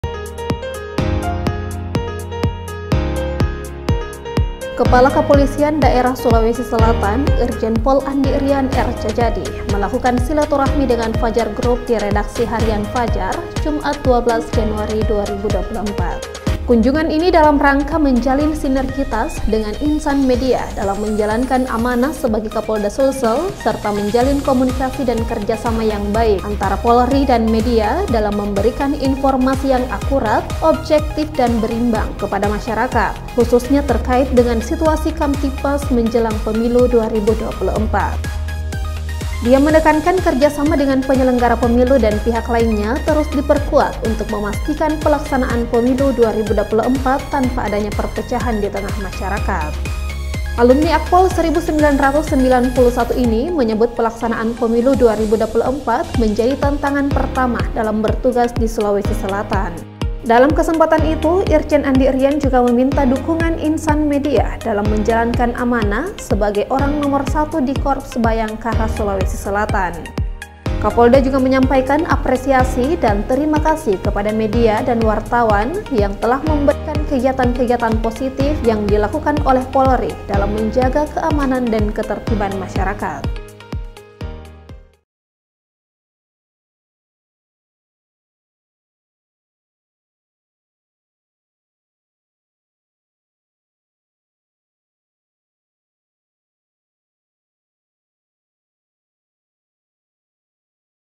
Kapolda Sulsel Jalin Silaturahmi dengan Fajar Group
Kepala Kepolisian Daerah (Kapolda) Sulawesi Selatan, Irjen Pol Andi Rian R Djajadi melakukan silaturahmi dengan Fajar Grup di Redaksi Harian FAJAR, Gedung Graha Pena, Makassar, Jumat (12/1/2024). Irjen Andi Rian menekankan Kerja sama dengan penyelenggara Pemilu dan pihak lainnya terus diperkuat untuk memastikan pelaksanaan Pemilu 2024 tanpa adanya perpecahan di tengah masyarakat.